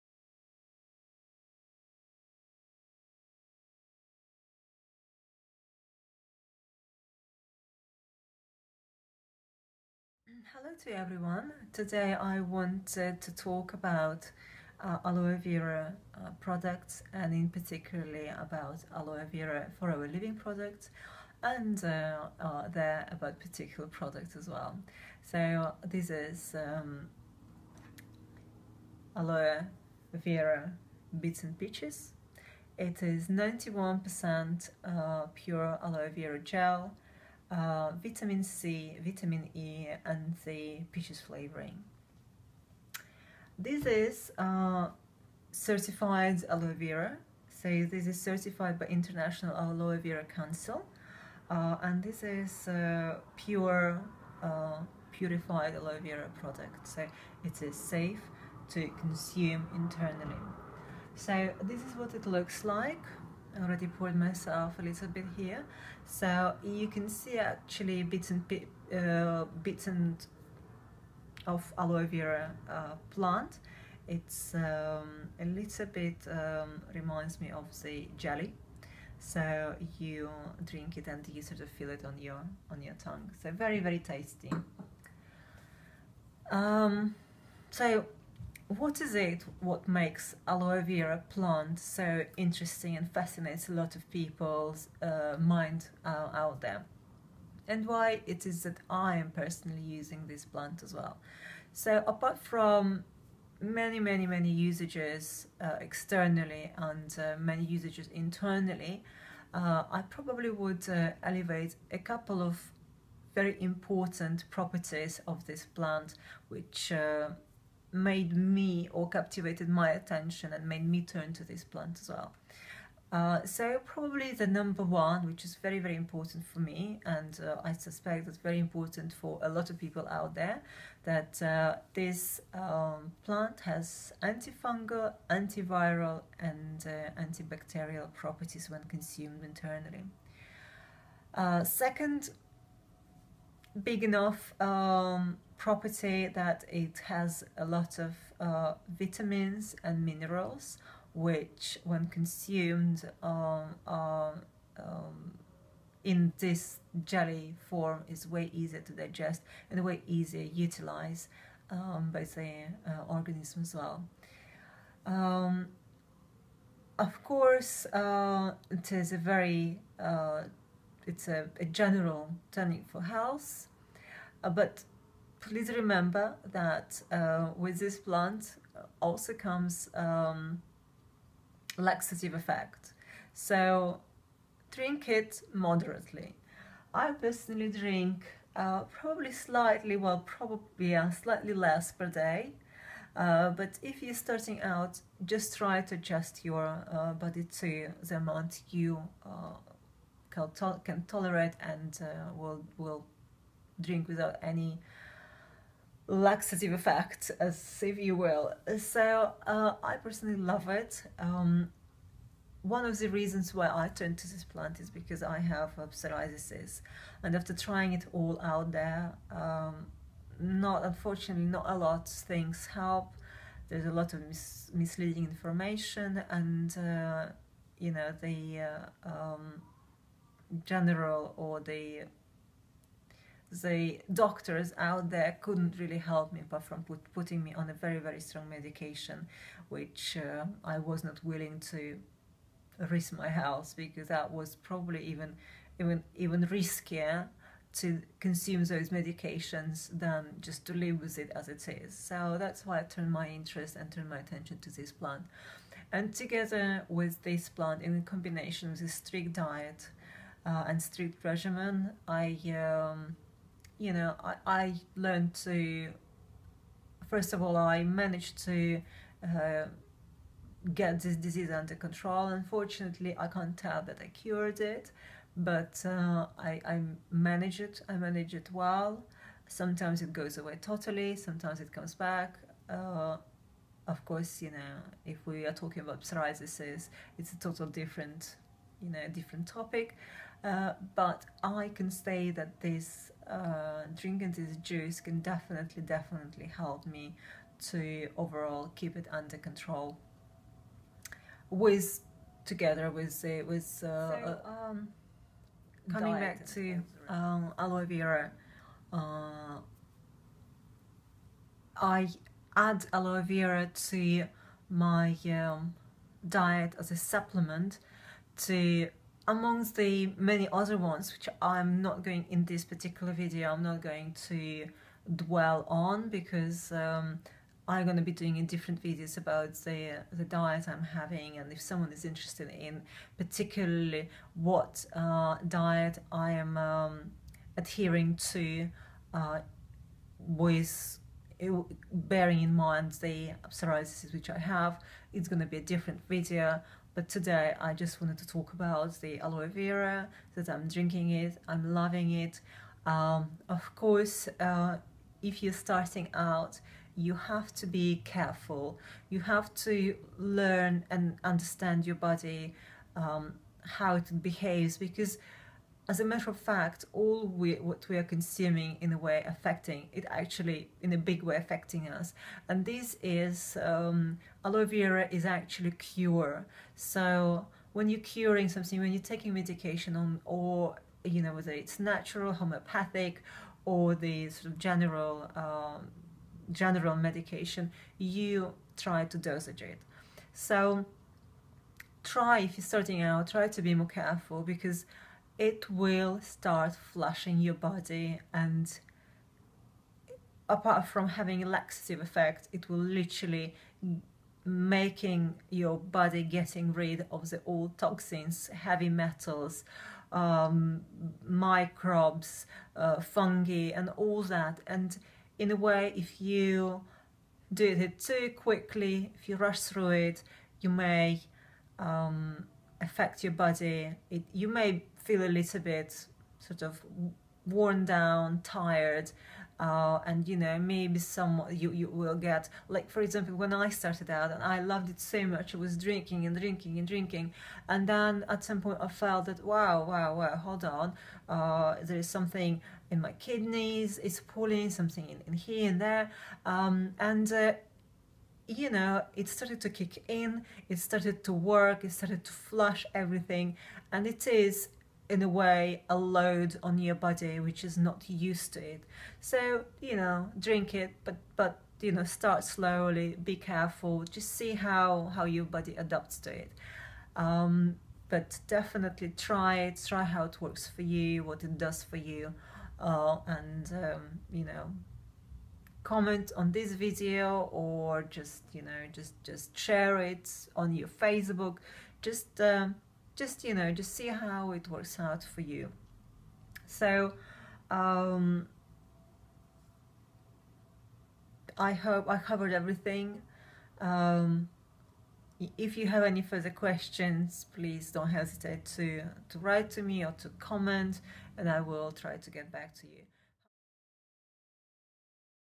Forever Living Aloe Vera Products Bits n' Peaches
Thành phần:

Nước uống dinh dưỡng Forever Aloe Bits N'peaches ®  Được làm từ Gel  Lô hội (có bao gồm các mảnh thịt lá) 91,043%, nước cốt đào tự nhiên 0,15%, Đường Fructose 7%, Axít Citric 0,25%, Kali Sorbate 0,095%, Axít Ascorbic 0,01%, Tocopherol 0,002% và hương đào tự nhiên 1,45%.

- Công Dụng:
+ Bổ sung 200 chất dinh dưỡng" 12 Vitamin, 20 khoáng chất, 18 Axit amin, Vitamin C & Tiền Vitamin A...
+ Có khả năng trung hòa các gốc tự do, ngăn ngừa bệnh ung thư.
+ Giảm đau, kháng viêm và chống lão hóa, giảm Stress.
+ Tái tạo tế bào và làm trẻ hóa tế bào.
+ Bồi bổ cơ thể, Protein và khoáng chất.
+ Kháng sinh thiên nhiên, khả năng đào thải độc tố, kháng viêm, chống vi khuẩn và virut tăng cường sức đề kháng đối với cơ thể.
+ Khả năng nhuận tràng tốt, giúp hệ tiêu hóa.
+ Ngăn chặn sự lão hóa cơ thể.
- Đối tượng sử dụng:
+  Người đang suy nhược cơ thể & ăn uống kém hấp thu.
+ Người làm việc nhiều, thức khuya, hút thuốc và uống rượu nhiều.
+ Người bệnh về nhiệt, táo bón.
+ Người bị bệnh mất ngủ, hen suyễn, dạ dày khó tiêu & hệ hô hấp.
- Cách dùng:
Lắc đều trước khi dùng. Ngày dùng 2-3 lần (50-60ml/lần), uống sau khi ăn. Bỏ vào tủ lạnh sau khi mở niêm phong để bảo quản.
 - Xuất xứ:
Forever Living Products (Công ty Aloe Vera of America, Inc. 13745 Jupiter Road Dallas, Texas 75238, Hoa Kỳ)
Để biết thêm thông tin về các sản phẩm tuyệt vời khác từ Forever vui lòng truy cập vào trang